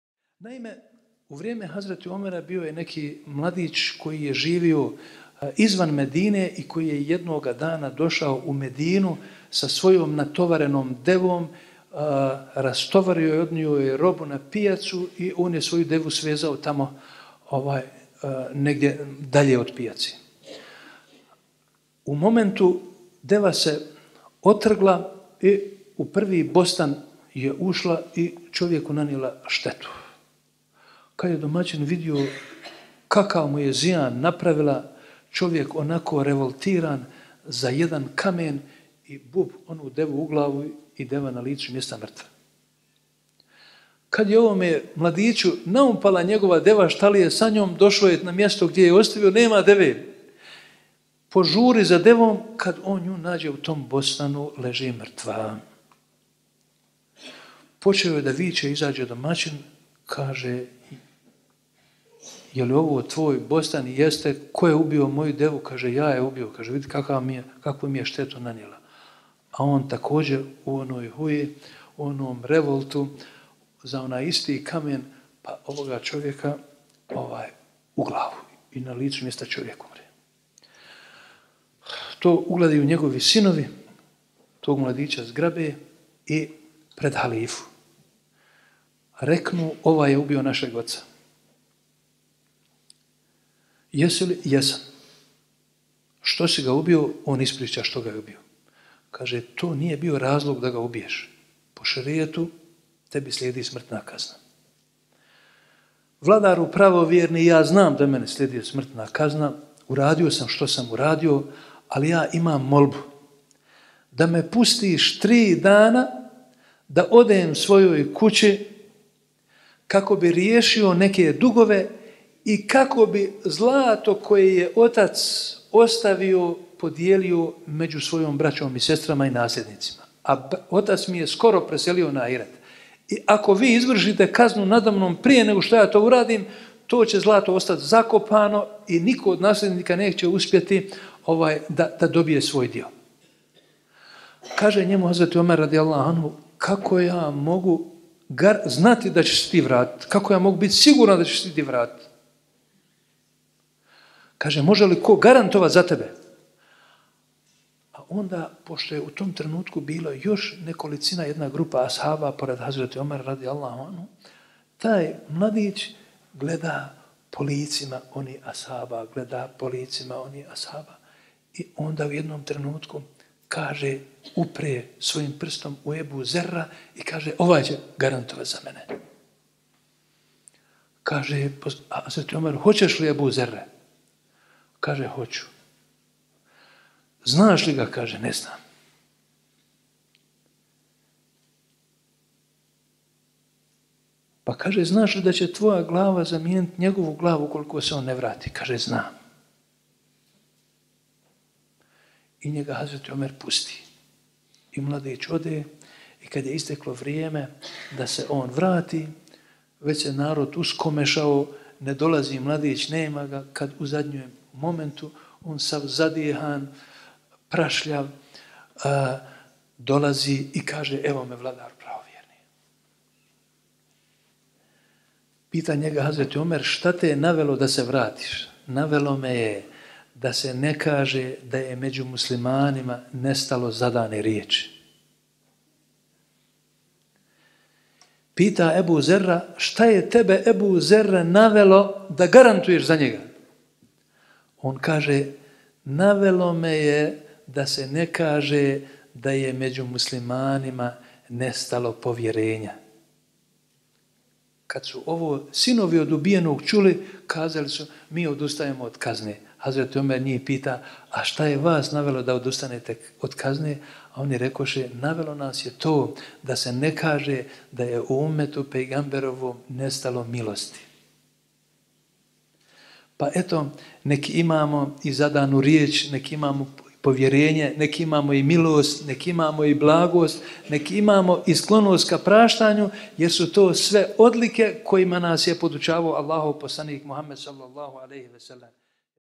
Dr. hfz. Halil Mehtić - Fenomenalna hikaja iz vremena Omera r.a.
U sklopu manifestacije "Selam, ya Resulallah", predavanje je održao uvaženi dr. hfz. Halil ef. Mehtić na temu "Muhammed, a.s., kao paradigma savršenog odgajatelja."  Na samom kraju predavanja hafiz Mehtić je ispričao sjajnu hikaju iz vremena Omera r.a koja obiluje sjajnim primjerima o datoj riječi, povjerenju, oprostu i pravednosti.